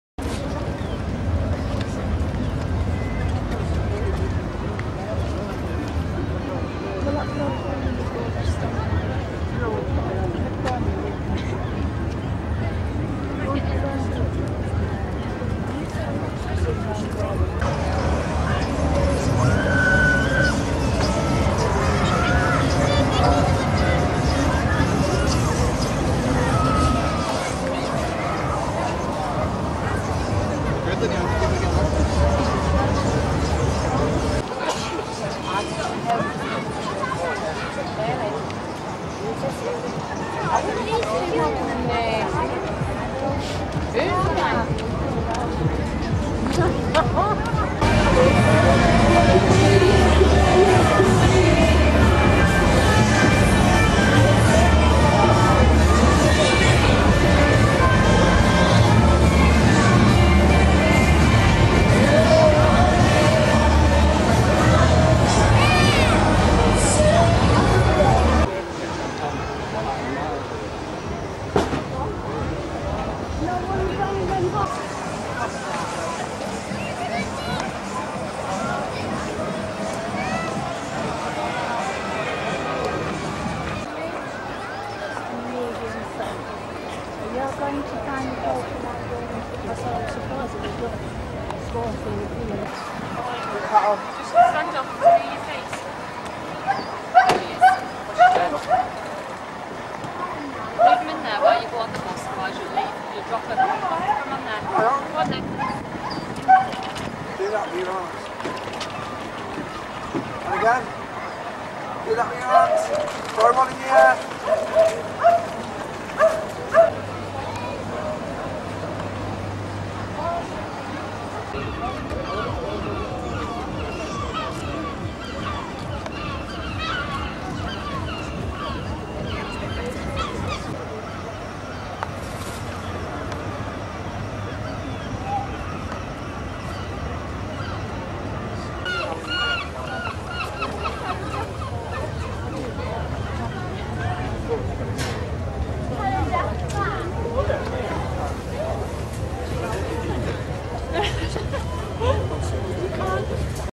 Sightseeing in Hartlepool - 7/8/10
Day out to Hartlepool to see the tall ships.